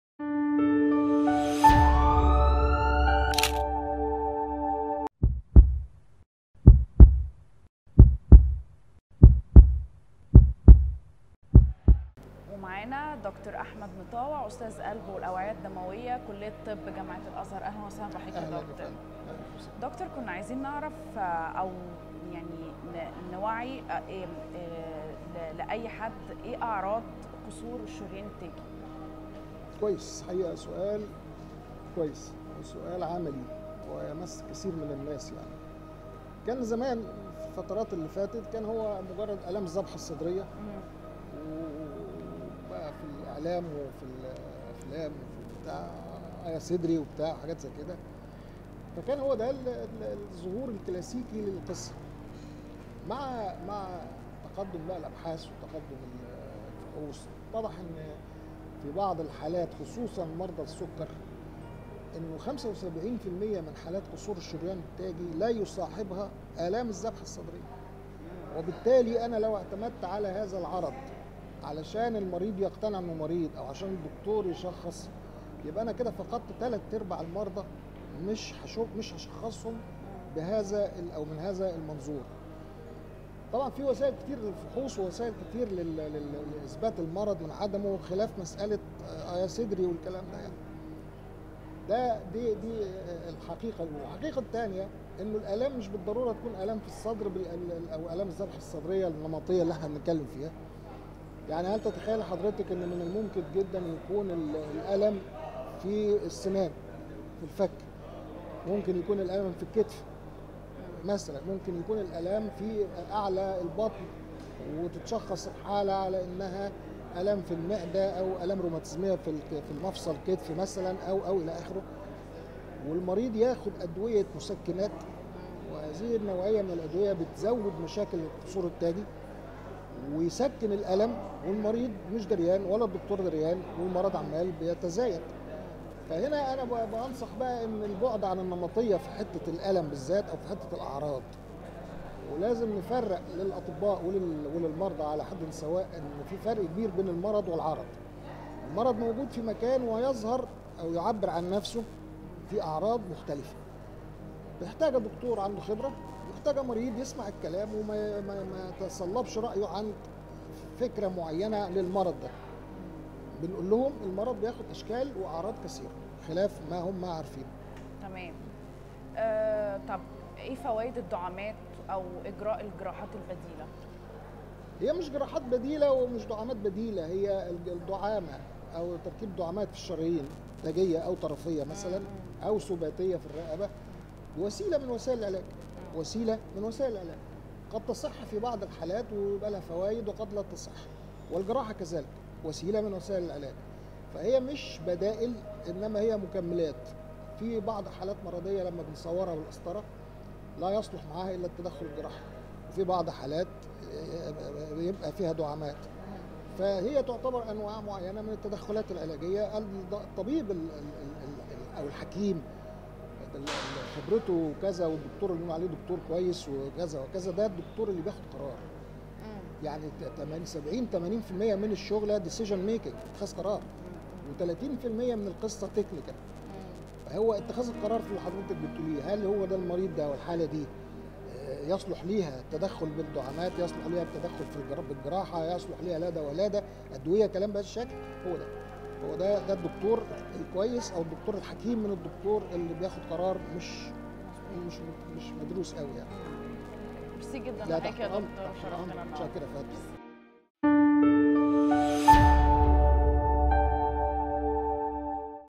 يتحدث عن أعراض قصور الشريان التاجى - دكتور أحمد مطاوع
الاستاذ الدكتور احمد مطاوع
استاذ القلب و الاوعية الدموية كلية الطب جامعة الازهر